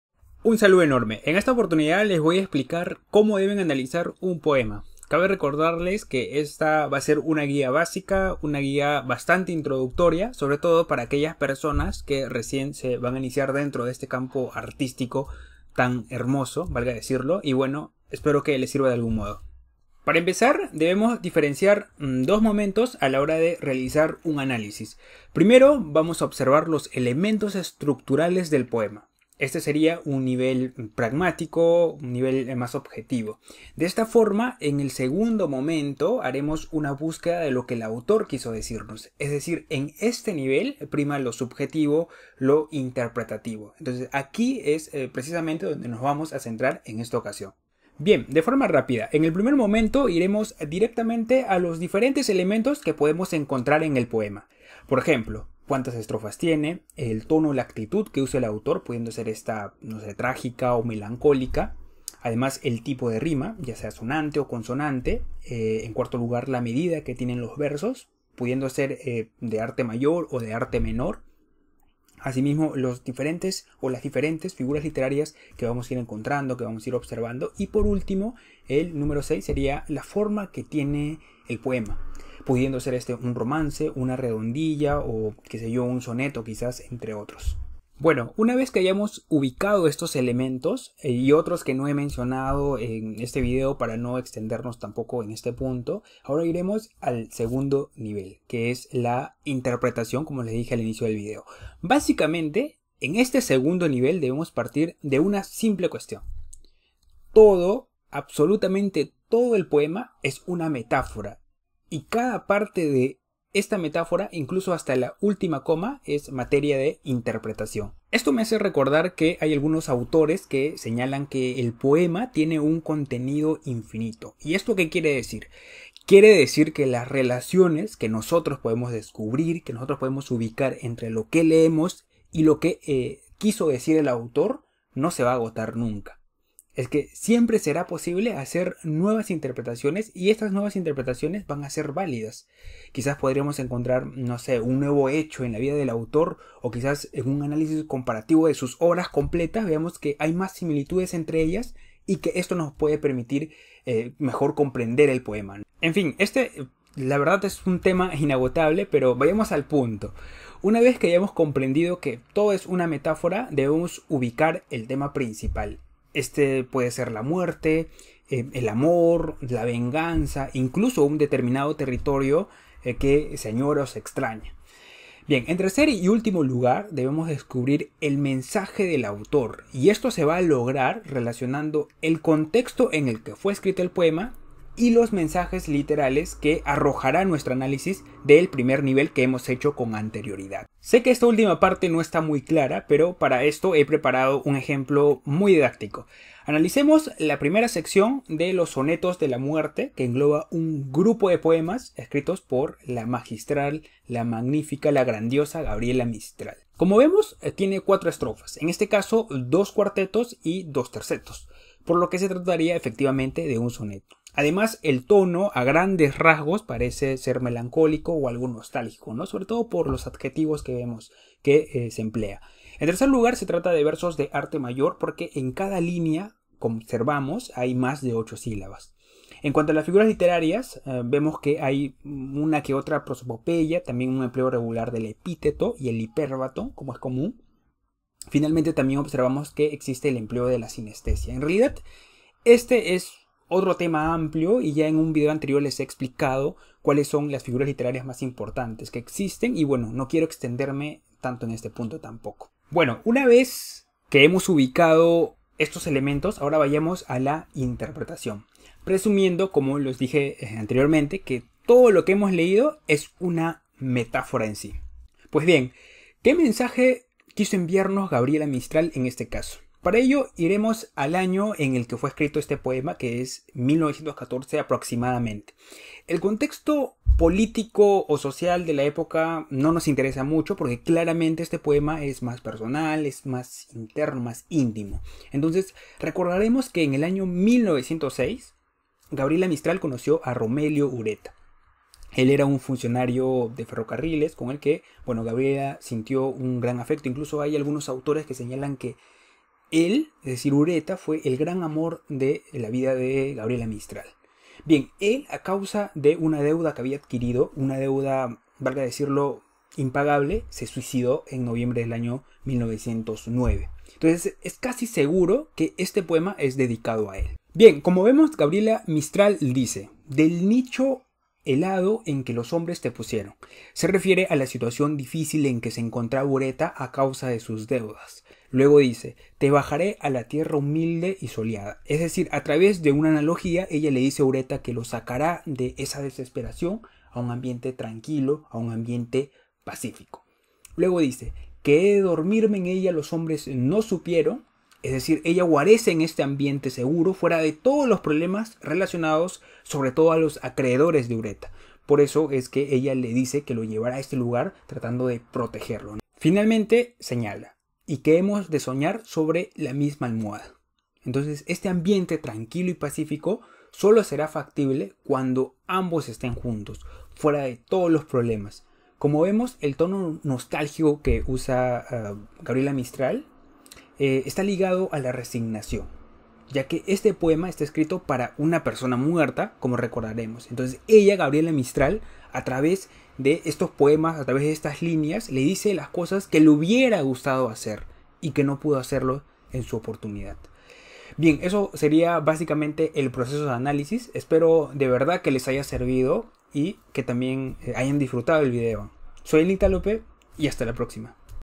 Cómo analizar (o interpretar) un poema en 2 pasos + ejemplo
Un cordial saludo a todos, en esta ocasión les explicaré como deben analizar un poema de forma sencilla; desde el estudio de sus métricas, hasta la misma interpretación. Espero, de todo corazón, que les sirva el video y estaré atento a cualquier pregunta que tengan. Abrazos!